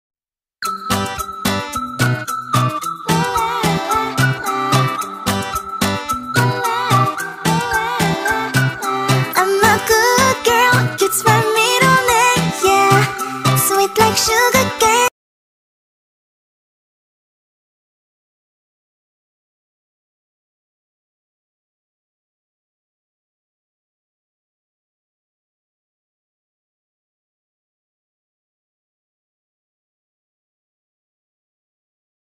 welsh pembroke corgi the good girl
welsh pembroke corgi the good girl. She likes to be pet. Bubbles is a very smart corgi dog that always demand to be pet.